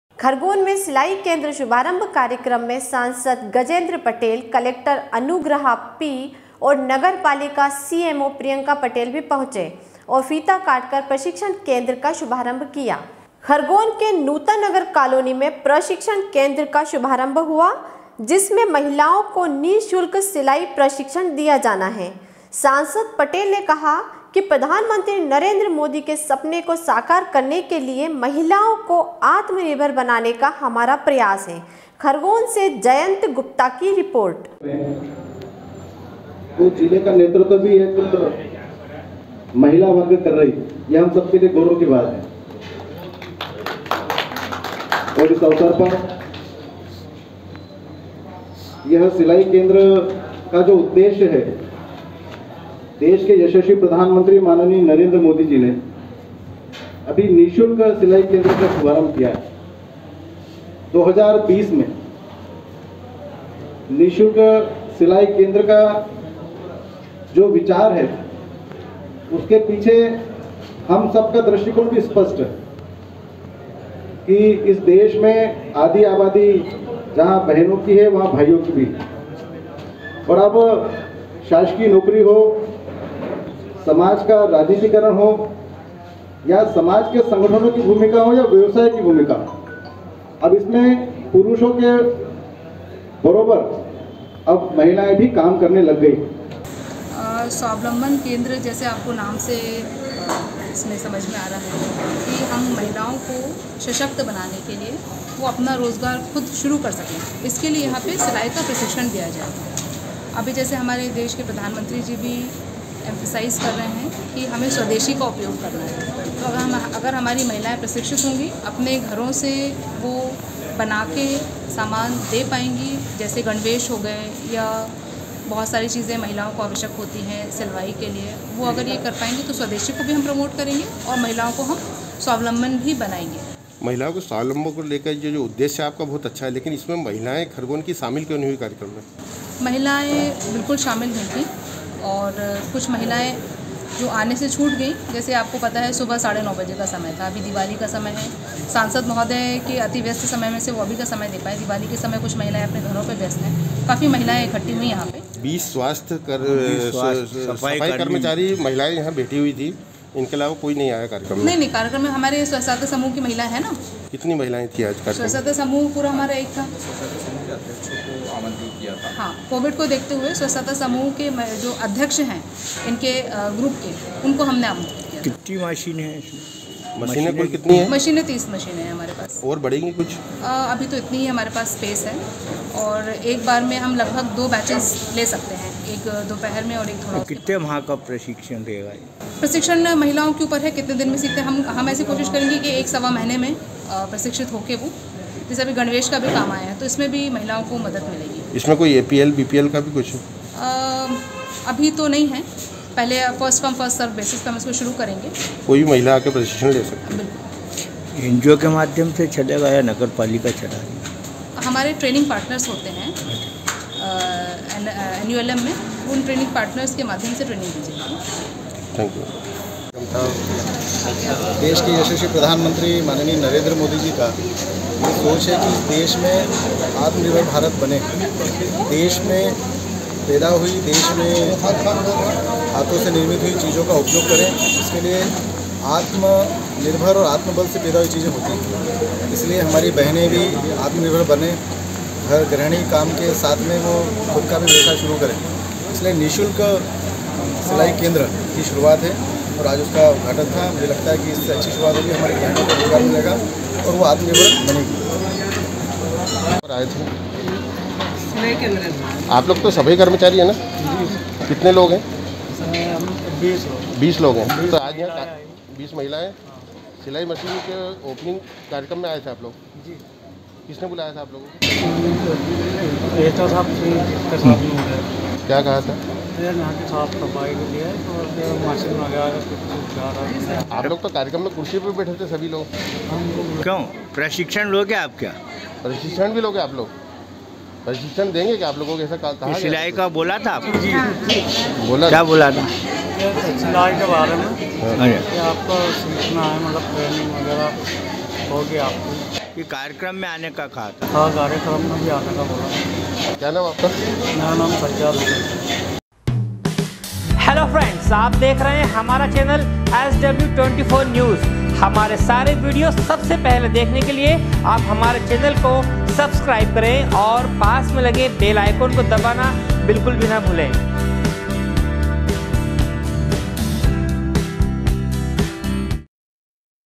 SW 24 News Madhyapradesh
खरगोन में सिलाई केंद्र शुभारंभ कार्यक्रम में सांसद गजेंद्र पटेल, कलेक्टर अनुगृहा पी और नगर पालिका सीएमओ प्रियंका पटेल भी पहुंचे और फीता काटकर प्रशिक्षण केंद्र का शुभारंभ किया। खरगोन से जयंत गुप्ता की रिपोर्ट